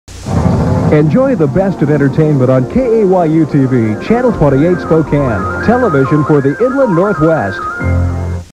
KAYU Station ID 1985
This is KAYU's station ID from 1985
KAYU is located in Spokane, WA